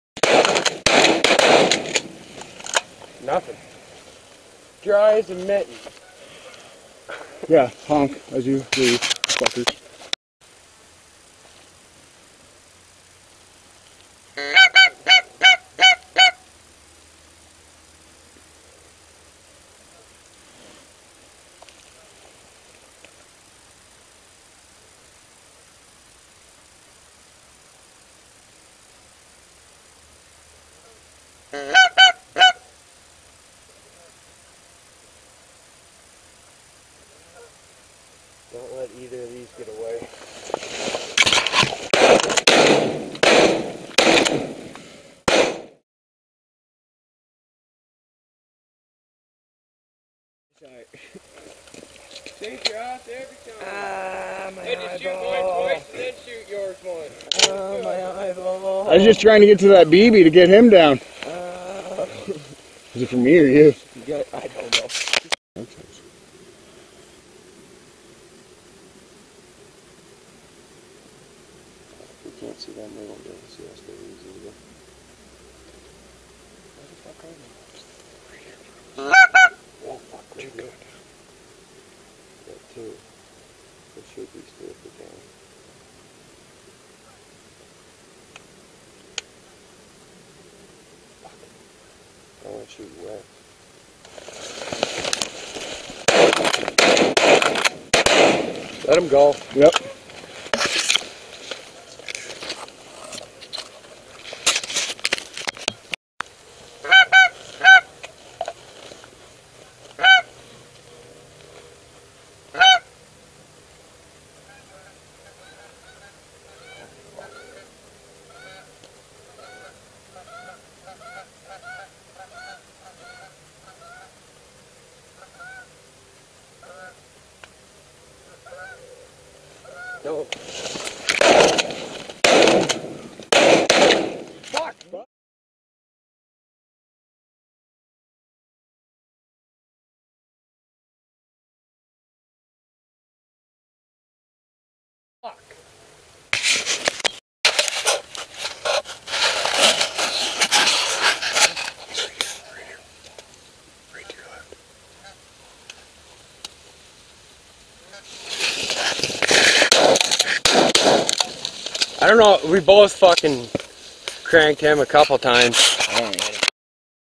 goose hunting southwestern ontario
goose hunting in cornfield in southwestern Ontario, couldnt get the camera working for the best parts